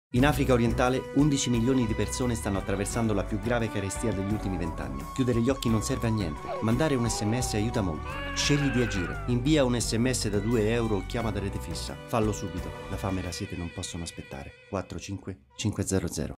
SPOT CARESTIA SOMALIA_APPELLO AGIREmp4
15 SECONDI DI APPELLO AGIRE PER RICORDARE LA CARESTIA IN SOMALIA E LA CRISI IN AFRICA ORIENTALE